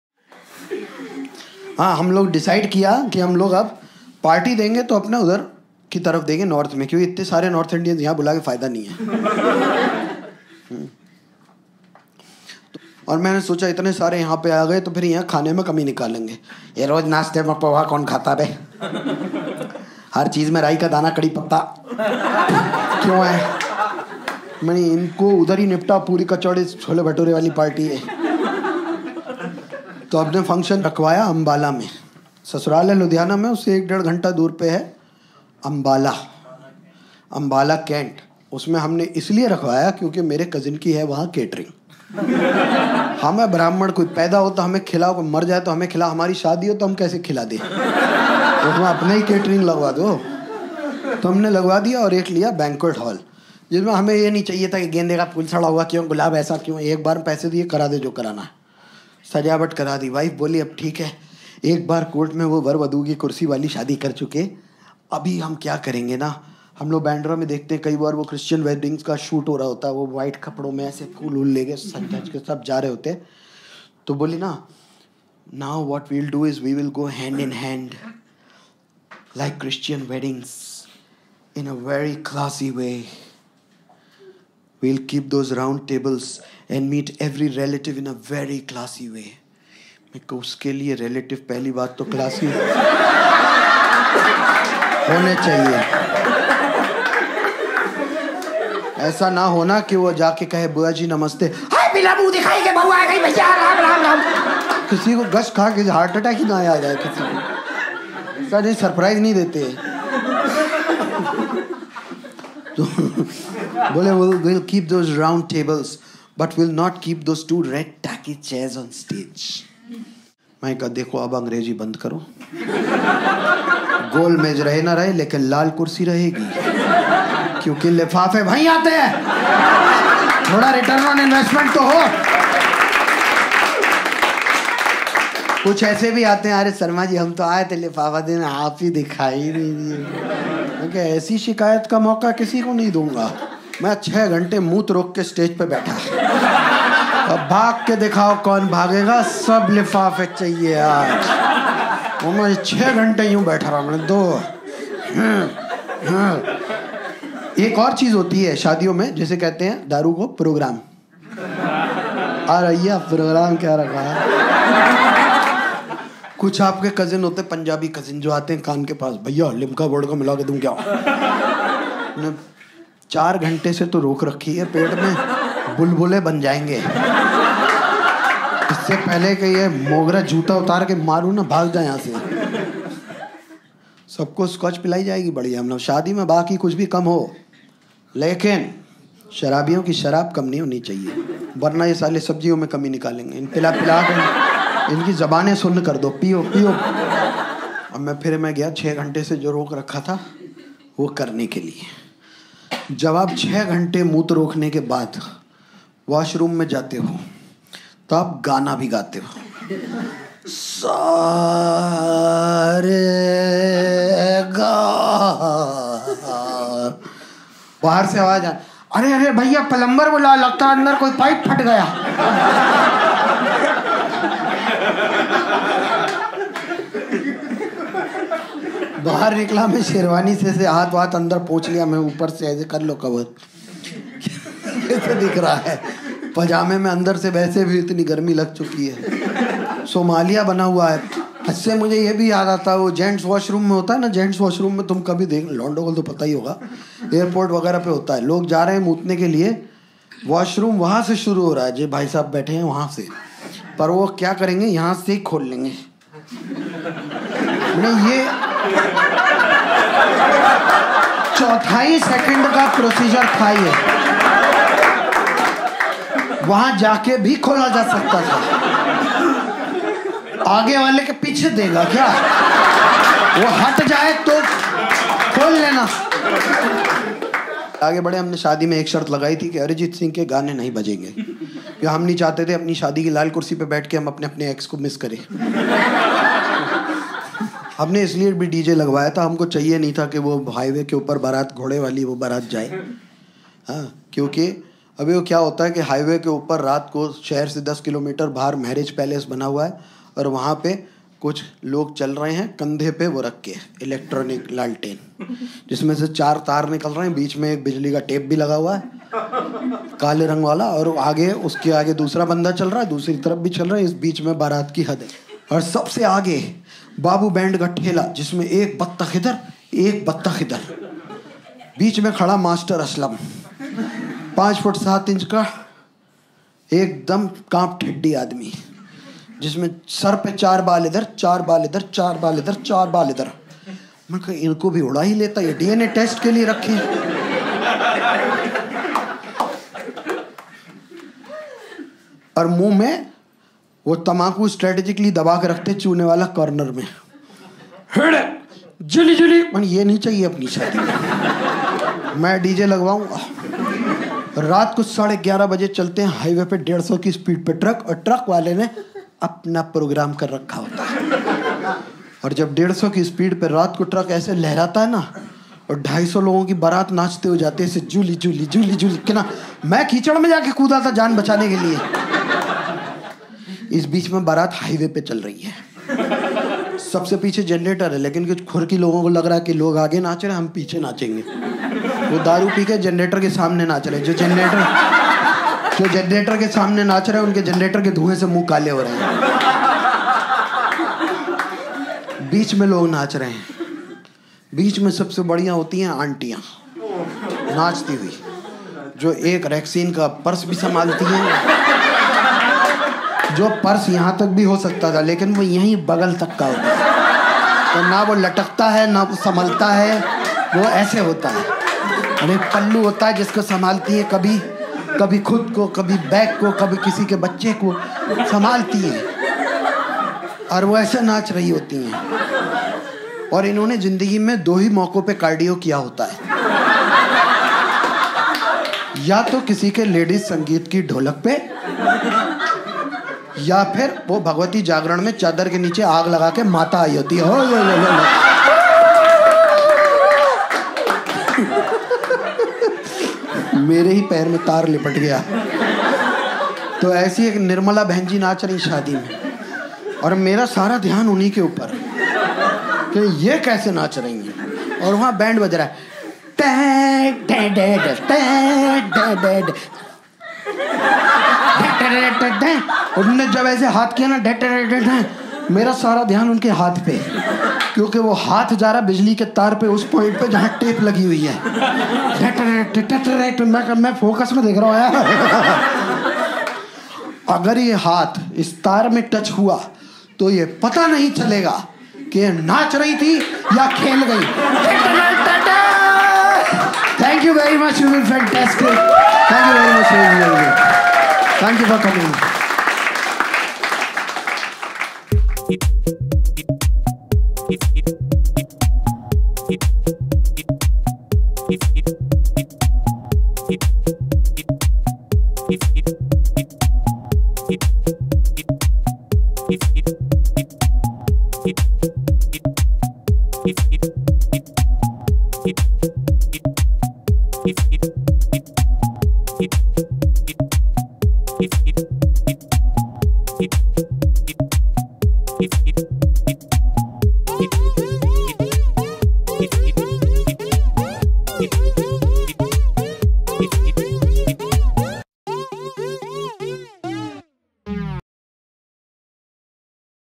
Nirmala Bhenji ka Dance - Sundeep Sharma Stand-up Comedy
This is the last video of my previous special, here I talk about our wedding reception party and a baraat that I had attended in U.P.  I am aware that the atmosphere all around is negative, I hope that this video will make you laugh and forget your worries for sometime. Stay Safe and Take care.

Recently acted in a sketch by @Jaspreet Singh  please watch it on his channel.
Thanks to @Anubhav Singh Bassi for suggesting the name Nirmala Bhenji

Credits:
Post Production: Myoho Films
Edit & Grade: Karan Asnani
Venue courtesy: The Habitat-Comedy and Music Cafe
Hospitality: Balraj Singh Ghai
Directed by: Dishang Popat
Shot by: (Team Habitat) Dishang Popat, Vivian Castelino Harsh Shah and Karan Kundnani
Live sound recording: Yash Rajiv Barse, Siegen Moopnar, Swarada Bodas
Sound mixing and mastering: Sreejith Menon
Written and performed by: Sundeep Sharma
Special Thanks to my wife: Dr. Ishmeet Nagpal